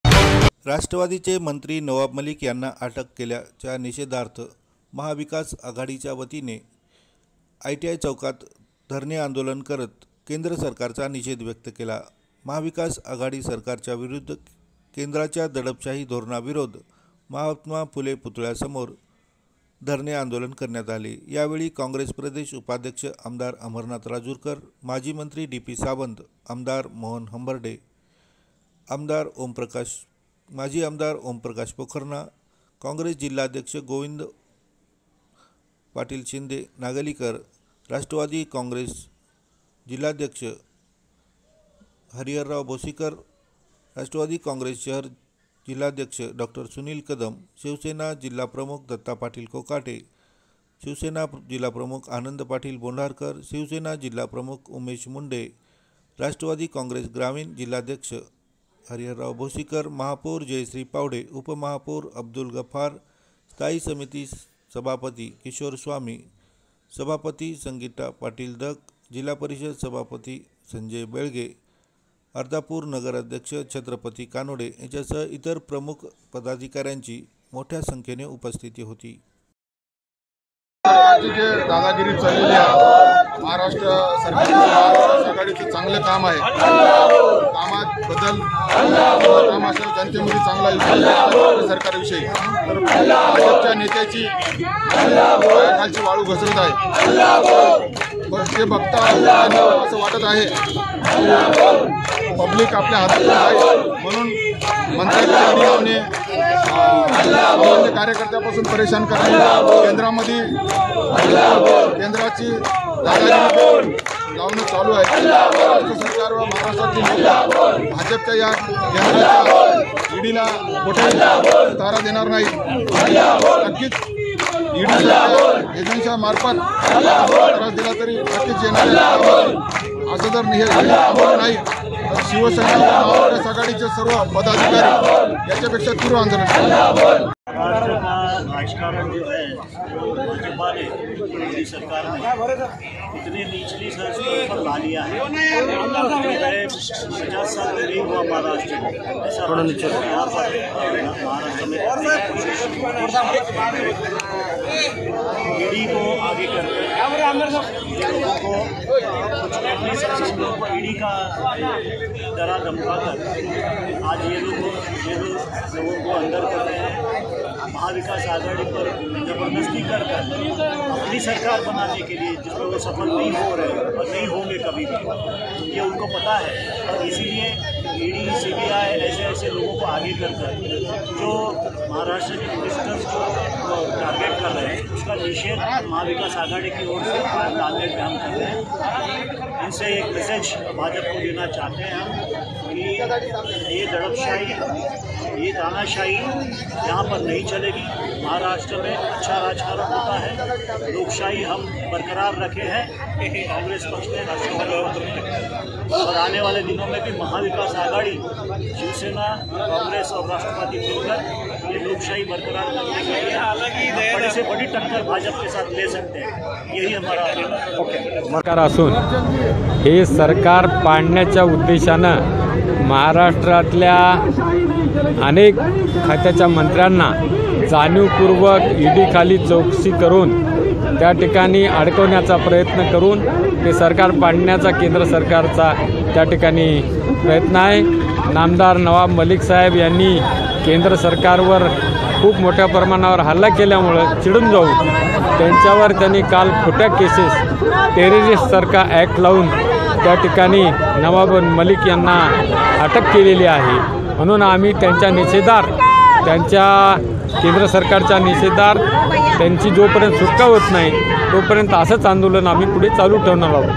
NANDED|मंत्री नवाब मलिक यांच्या अटकेच्या निषेधार्थ महाविकास आघाडीचे धरणे आंदोलन
NEWS || LIVE || UPDATE || BREAKING || NAMASKAR MAHARASHTRA
|मंत्री नवाब मलिक यांच्या अटकेच्या निषेधार्थ महाविकास आघाडीचे धरणे आंदोलन